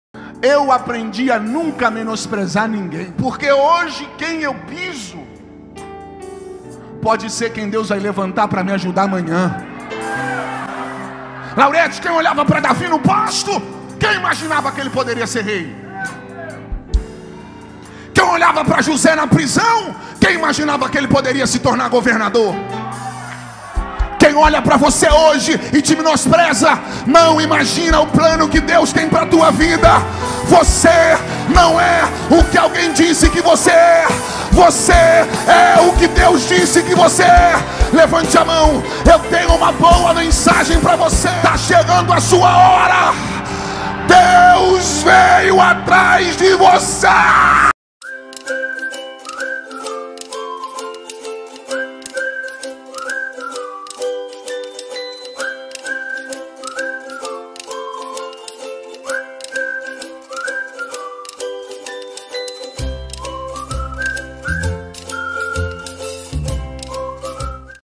PASTOR FALOU TODA A VERDADE E VEJA A REAÇÃO DA IGREJA!
Eu aprendi a nunca menosprezar ninguém; porque hoje quem eu pizo, pode ser quem Deus vai levantar para me ajudar amanhã. Laurete, Quem olha para Davi no pasto quem imaginava que ele poderia ser rei? Quem olhava
pra José na prisão, quem poderia imaginar que ele poderia se tornar governador? Quem olha pra você hoje e te menospreza não imagina o plano que Deus e pra tua vida. Você não é o que alguém disse que você é. Você é o que Deus disse que você é! Levante a mão, eu tenho uma boa mensagem pra você, tá chegando a sua hora. Deus veio atrás de você...
Nos ajude a pregar o Evangelho compartilhando o canal. Deus te abençoê!

✅Assista este e outros vídeos do canal.